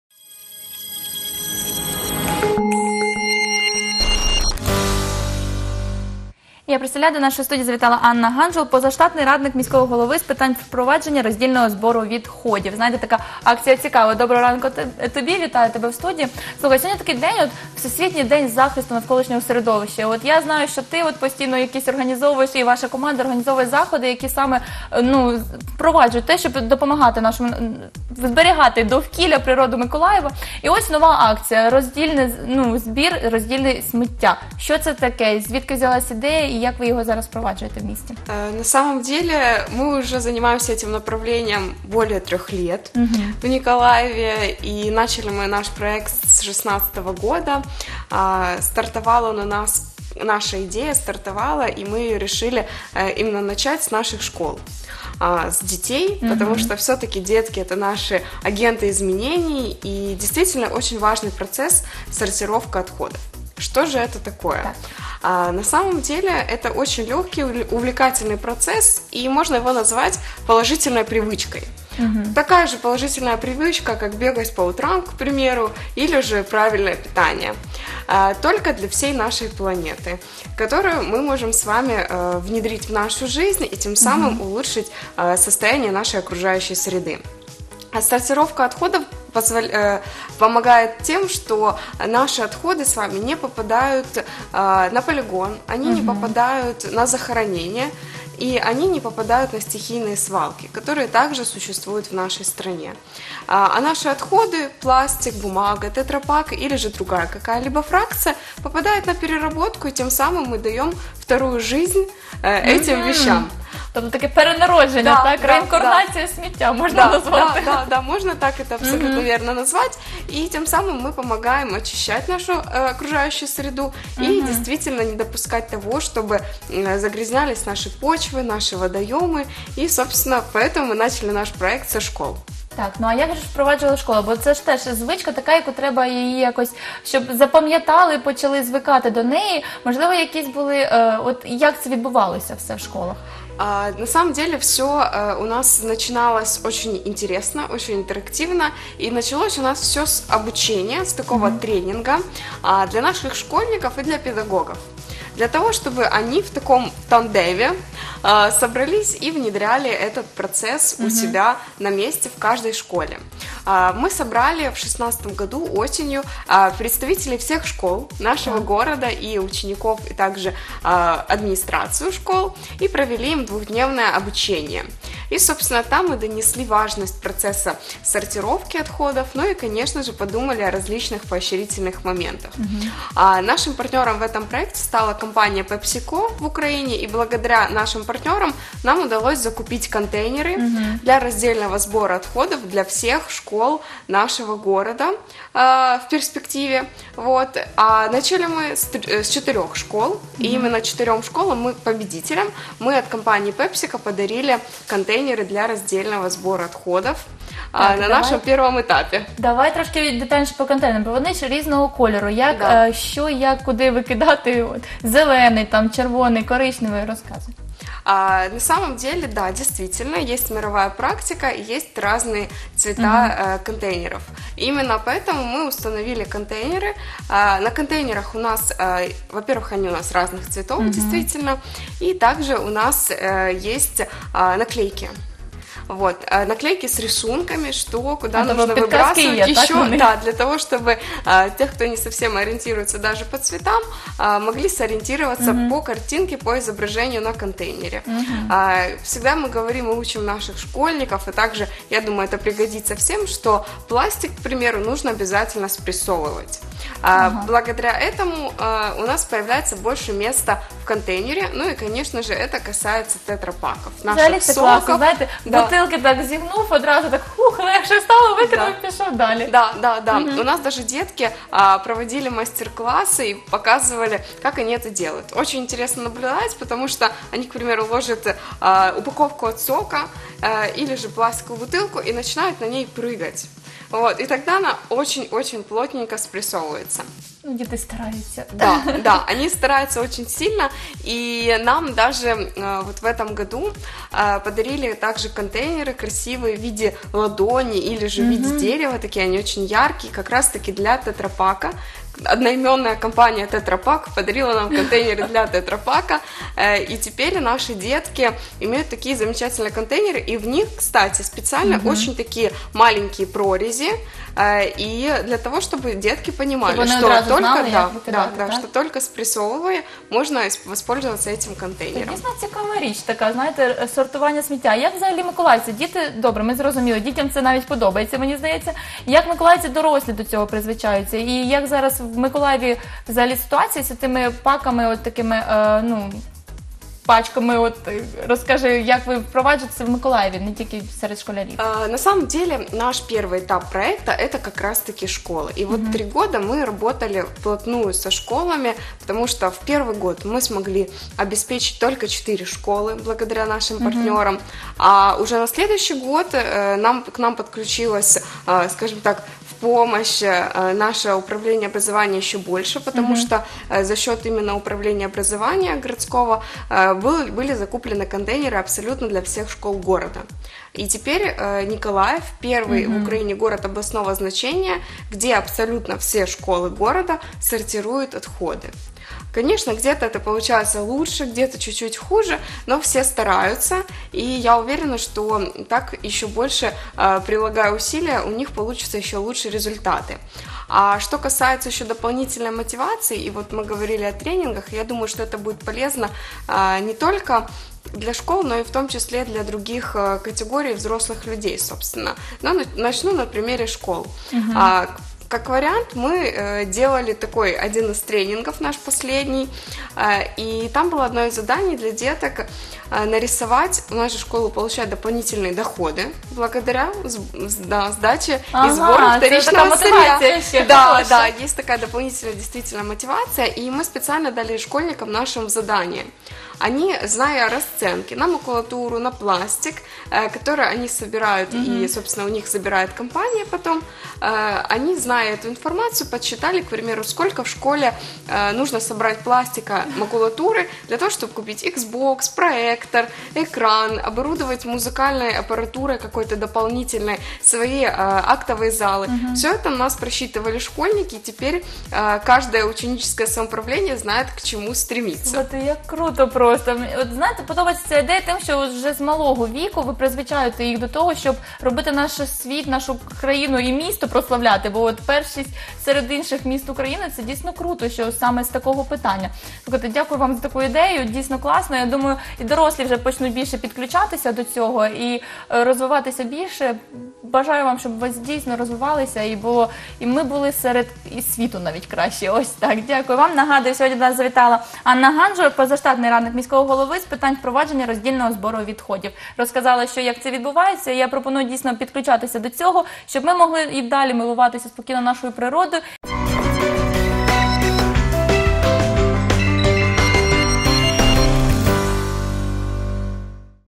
5.06.18. Новий день. Сортуємо сміття в школах
Сортування сміття дає можливість повторно використовувати вторинну сировину. Яка користь від цього та чи готові миколаївці до такого способу захисту навколишнього середовища, дізналися від гості студії Анни Ганжул.